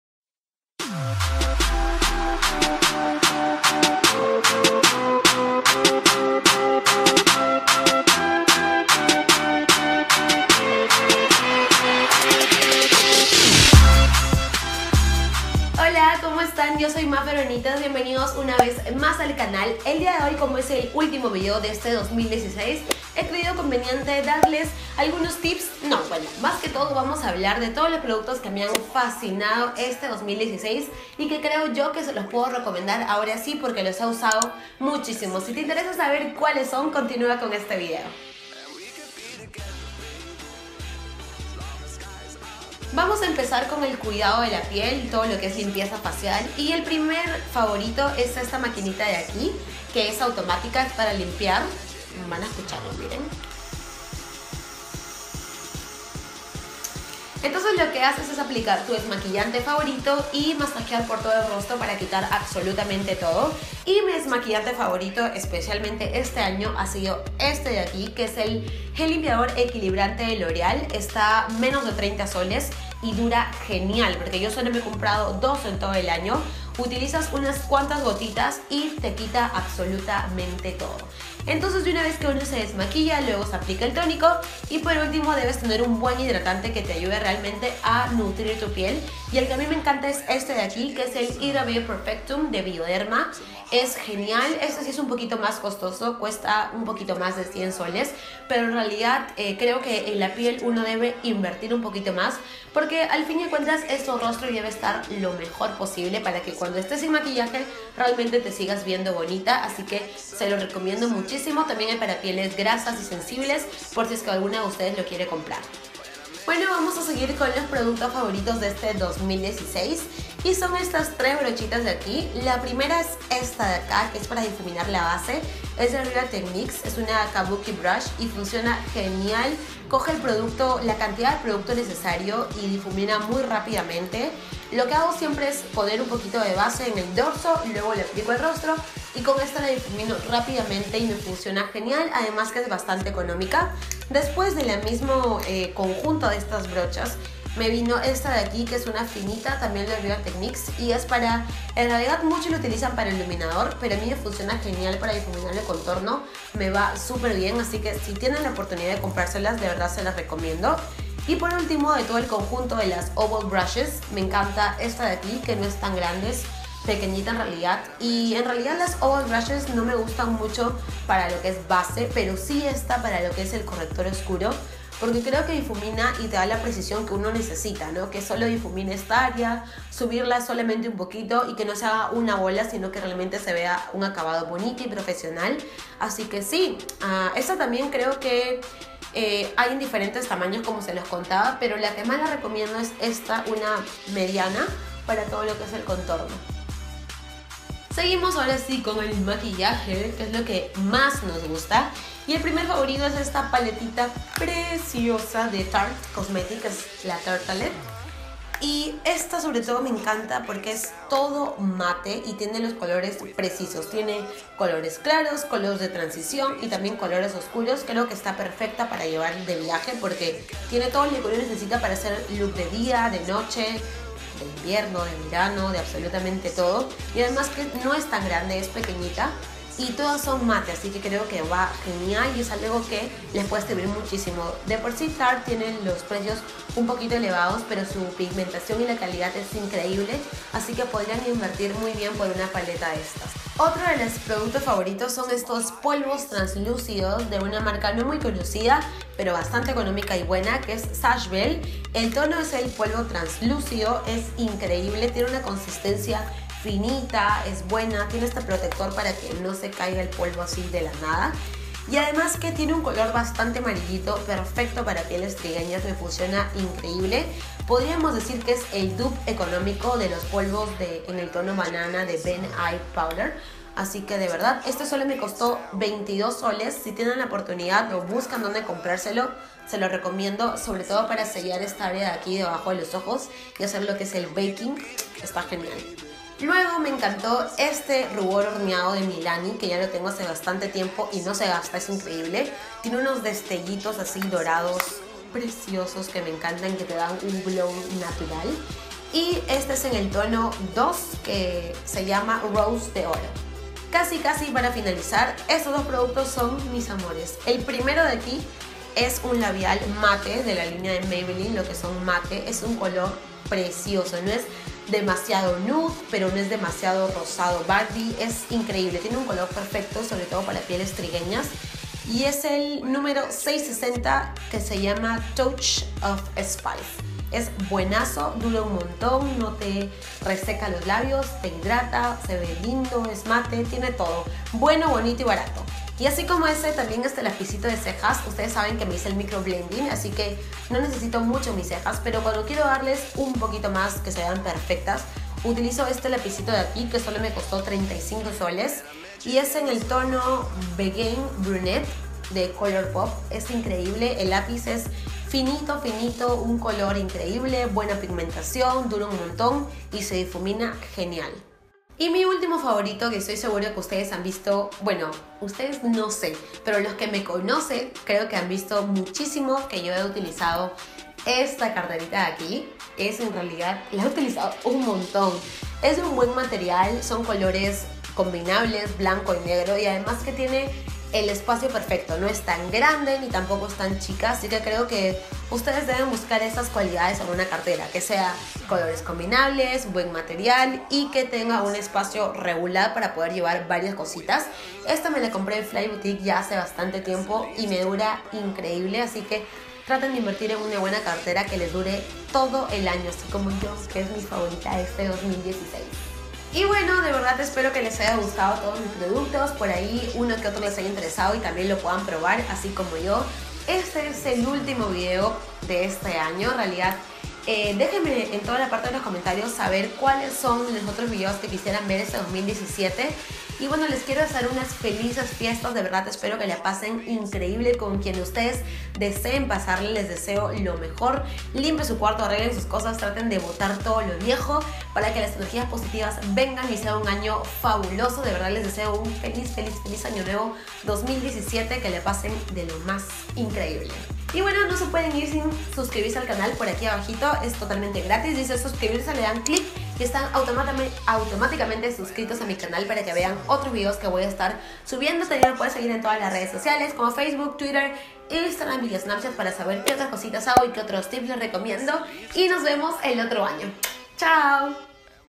LO MEJOR DEL 2016 - MAQUILLAJE BELLEZA Y MÁS !! / Mafer Benites
Holaa preciosas estamos a muy poquito de acabar este año 2016 ! Así que por aquí les muestro los productos que me han encantado este año y recomiendo !!!

maferbenites

Cámara y Post producción:
José Luis Benites. Mira su página: